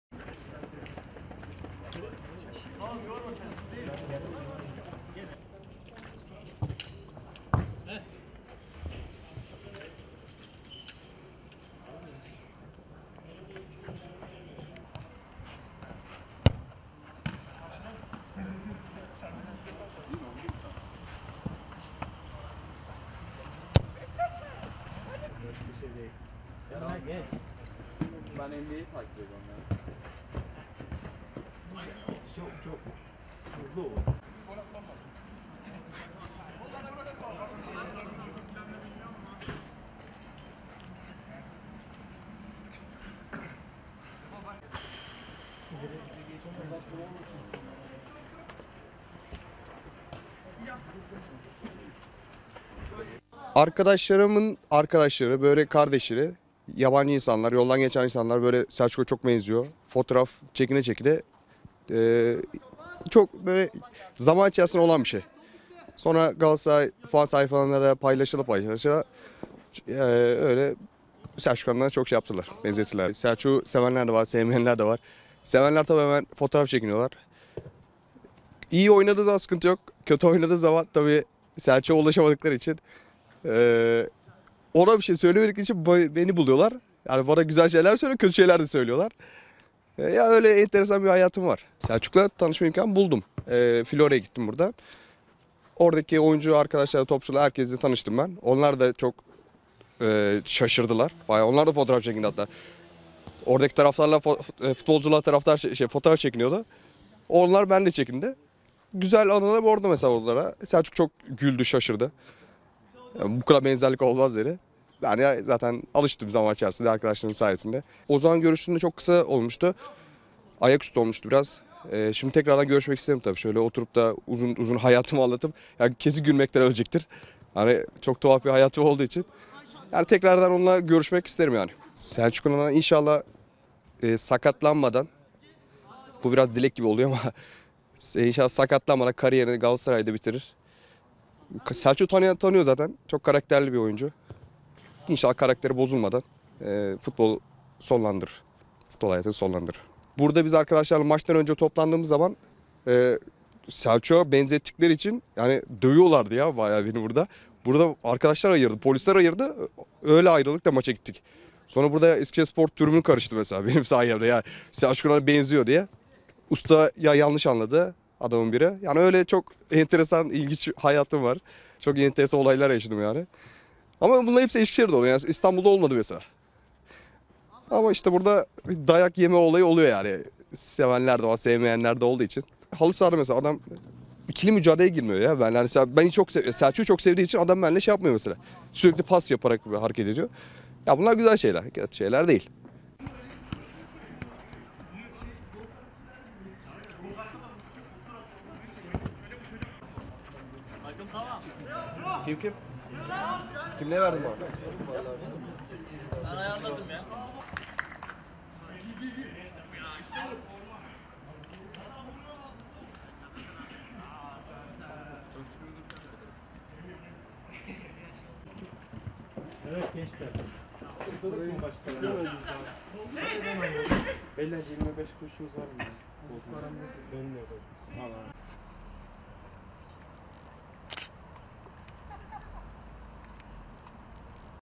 Eskişehirli Selçuk İnan görenleri şaşkına çeviriyor
Eskişehir’de bir fabrikada işçi olarak çalışan 26 yaşındaki Uğur Uçak, milli oyuncu Selçuk İnan’a benzerliği ile görenleri şaşırtıyor.
Zaman içerisinde çevresindeki insanlar tarafından Selçuk İnan’a benzerliği ile dikkat çeken Uğur Uçak'ın tanınırlığı gün geçtikçe artıyor. Bir fabrikada işçi olarak çalışan ve benzerliği sayesinde Galatasaraylı futbolcular ile de tanışan Uçak, futbolcuların bile kendisini görünce çok şaşırdığını ifade etti. Galatasaray tesislerinde kendisini Selçuk İnan’a benzeten Muslera ve Serdar Aziz ile de fotoğraf çekinen 26 yaşındaki Uğur Uçak, zaman zaman benzerliği yüzünden deneyimli futbolcuya verilen tepkilerin kendisine de verildiğini dile getirdi.

“Yoldan geçenler Selçuk’a çok benziyor diye fotoğraf çekiniyor"
Selçuk İnan ile benzerliğinin önce çevresi sonra tanımadığı insanlardan gelen tepkiler ile farkına varıldığını ifade eden Uğur Uçak, “Arkadaşlarımın arkadaşları, kardeşleri, yabancı insanlar, yoldan geçenler Selçuk’a çok benziyor diye fotoğraf çekine çekine zaman içerisinde olan bir şey. Galatasaray fan sayfaları da paylaşa paylaşa Selçuk İnan’a çok benzettiler. Selçuk’u sevenler de var sevmeyenler de var. Sevenler hemen fotoğraf çekiniyorlar. İyi oynadığı zaman sıkıntı yok. Kötü oynadığı zaman Selçuk’a ulaşamadıkları için ona bir şey söylemek için beni buluyorlar. Bana güzel şeyler söylüyorlar, kötü şeyler de söylüyorlar. Enteresan bir hayatım var” şeklinde konuştu.

“Selçuk çok şaşırdı, ‘Bu kadar benzerlik olamaz’ dedi”
Galatasaray antrenmanını izlemeye gittiğini ve bu antrenmanda taraftarların futbolcular ile fotoğraf çekinirken birden kendisi ile de fotoğraf çekinmek isteyenlerin olduğunu dile getiren Uçak, “Selçuk ile tanışma imkanı buldum. Buradan Florya’ya gittim. Oradaki oyuncu arkadaşlarla tanıştım. Onlar da çok şaşırdılar. Hatta onlar da fotoğraf çekindi. Oradaki taraftarlar futbolcular fotoğraf çekiniyordu. Onlar benimle çekindi. Güzel anılarım oldu. Mesela Selçuk çok güldü, şaşırdı. ‘Bu kadar benzerlik olmaz’ dedi. Ben zaman içerisinde arkadaşlarım sayesinde alıştım zaten. O zaman görüştüğümde çok kısa olmuştu, biraz ayaküstü olmuştu. Şimdi tekrardan görüşmek isterim. Oturup uzun uzun hayatımı anlatmak isterim. Kesin gülmekten ölecektir çok tuhaf bir hayatım olduğu için. Tekrardan onula görüşmek isterim. Selçuk İnan inşallah sakatlanmadan kariyerini Galatasaray’da bitirir. Selçuk’u tanıyan tanıyor zaten. Çok karakterli bir oyuncu. İnşallah karakteri bozulmadan futbol hayatını sonlandırır” ifadelerini kullandı.

“Selçuk’a benzettikleri için beni dövüyorlardı”
Benzerliği yüzünden zaman zaman olumsuz tepkiler aldığını da ifade eden 26 yaşındaki Uğur Uçak son olarak şu ifadeleri kullandı:
“Burada biz arkadaşlarla maçtan önce toplandığımız zaman Selçuk’a benzettikleri için beni dövüyorlardı. Arkadaşlar ve polisler ayırdı. Öyle ayrılıp maça gittik. Benim yüzümden Eskişehirspor tribünü karıştı. Selçuk İnan’a benziyor diye yanlış anladı adamın biri. Öyle çok enteresan, ilginç bir hayatım var. Çok enteresan olaylar yaşadım. Bunlar hepsi Eskişehir’de oldu, İstanbul’da olmadı. Dayak yeme olayı oluyor. Sevenler de var, sevmeyenler de olduğu için. Mesela halı saha da adam benimle ikili mücadeleye girmiyor. Selçuk’u çok sevdiği için benimle ikili mücadeleye girmiyor. Sürekli pas yaparak hareket ediyor. Bunlar güzel şeyler kötü şeyler değil.”